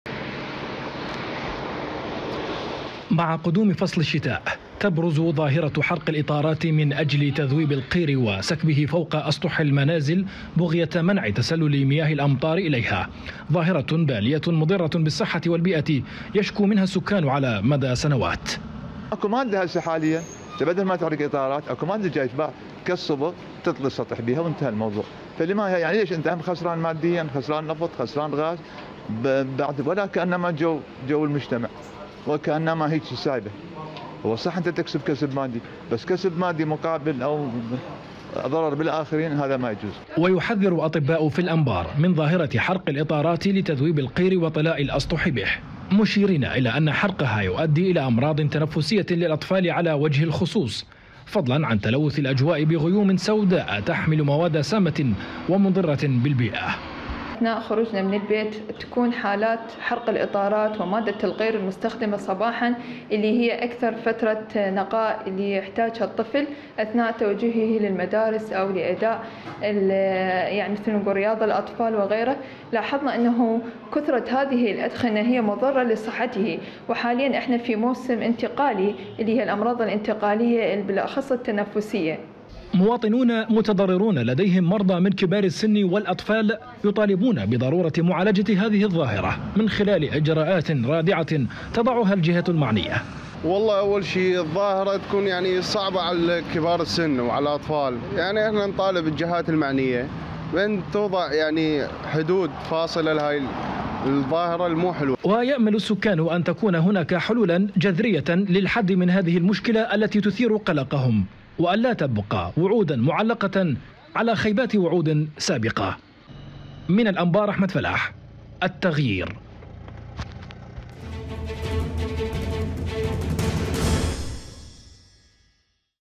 ظاهرة خطيرة تهدد الانبار .. وتحذير طبي عاجل | تقرير
طالب مواطنون في الانبار بوضعِ حدٍ لظاهرةِ احراق الإطارات في الشوارع والأحياءِ السكنية وما يَنتج عنها من أضرارٍ بيئية وصحية .. كما حذر أطباءُ من خطورةِ استنشاقِ الدخان الأسود الناتجِ عن احراق الإطاراتِ لاسيما لدى الأطفال وكبار السن. حيث تَبرز هذه الظاهرة مع قدومِ فصل الشتاء من أجلِ تذويب مادةِ القير وطلاءِ الأسطح بها لمنعِ تسللِ مياه الأمطار.
تقرير - أحمد فلاح
موقع قناة التغيير الفضائية  الالكتروني